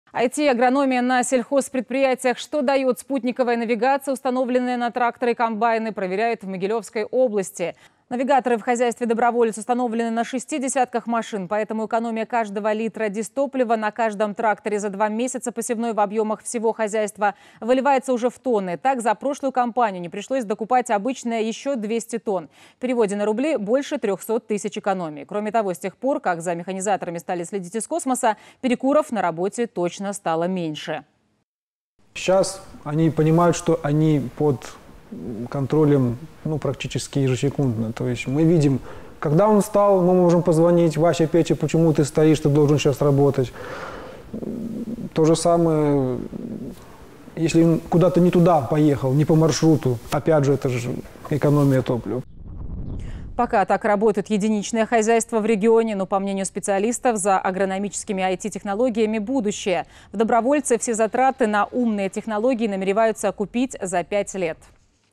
Что даёт спутниковая навигация, установленная на тракторы и комбайны?
Симбиоз IT и агрономии проверяют в Могилёвской области. В хозяйстве «Доброволец» установлены GPS-навигаторы на шести десятках машин. Поэтому экономия каждого литра солярки на каждом тракторе за два месяца посевной в объёмах всего хозяйства выливается уже в тонны.